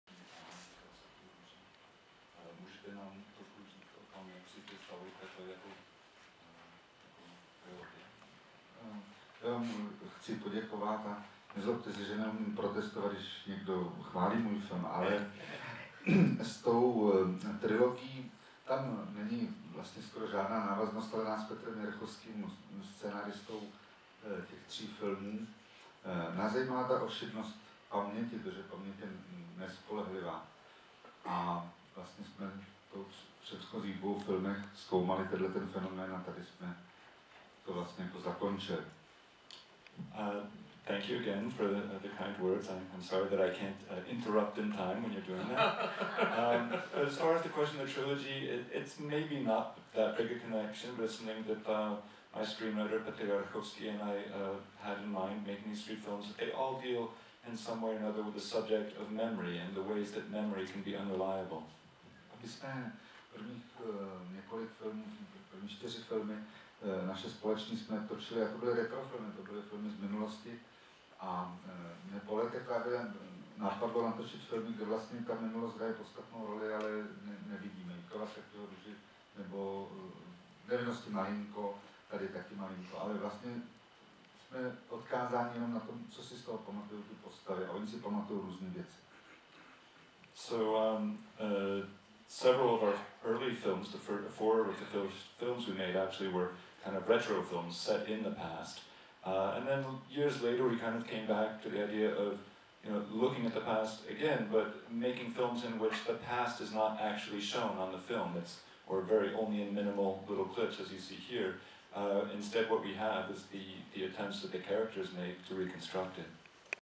CCNY - Panorama Europe Film Festival: Q&A with Jan Hrebejk (part I)
Museum of the Moving Image, Czech Center New York and the European Union National Institutes for Culture present the first annual edition of Panorama Europe in New York City to run from Friday, April 4--Sunday, April 13, 2014.
Q&A with director Jan Hrebejk. Screening of HONEYMOON (Libanky)